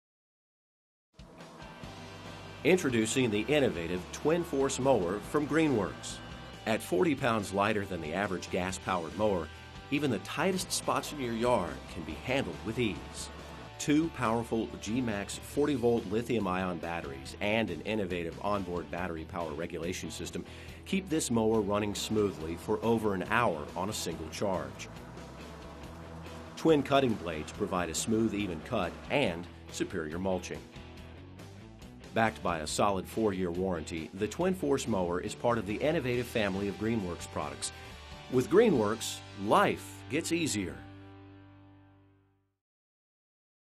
GreenWorks 25302 Twin Force 40V Cordless Lawn Mower Review
You know?, This is very cool looking and lightweight. Nice hard case clippings bag (but a little small). Nice easy height adjustment. I cannot comment on the longevity of the batteries as I have only had this mower a very short time, however as received they worked really well, charged quickly, and from a battery technology standpoint this seems to be a well executed concept. Auto battery selection while mowing is seamless. My wife like it very much, sometimes she do this job by herself. It is the best lawn mower ever. Nice try Greenworks

I WOULD LIKE TO RECOMMEND TO YOU ALL!!!